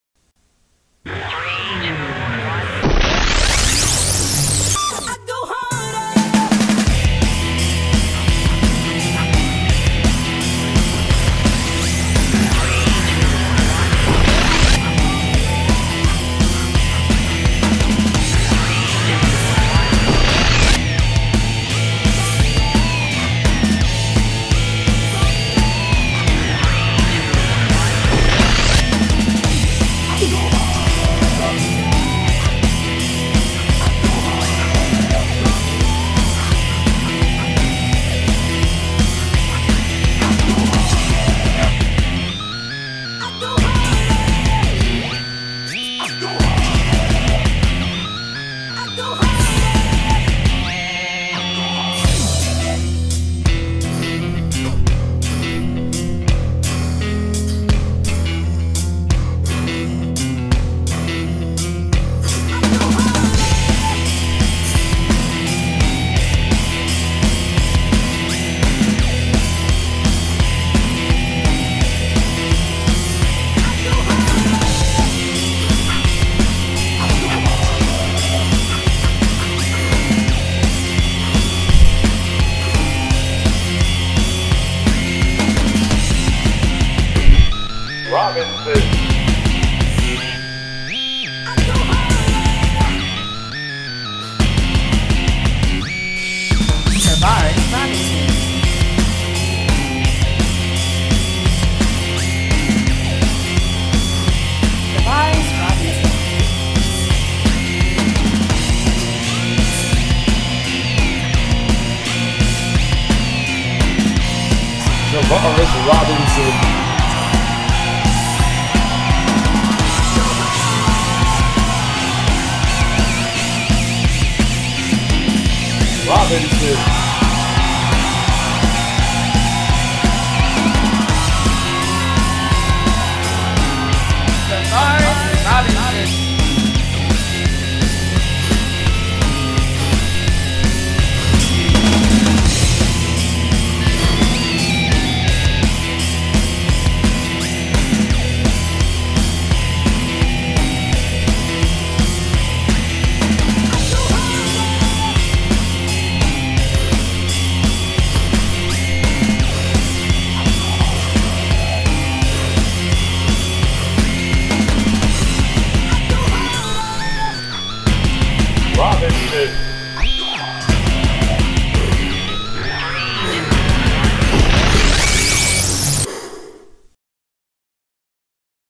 Travaris Robinson - H.S. Football Highlights REMIX
This video features Travaris Robinson's high school football highlights.
Miami Coral Park- 1998.

Production by CSCOPE2020.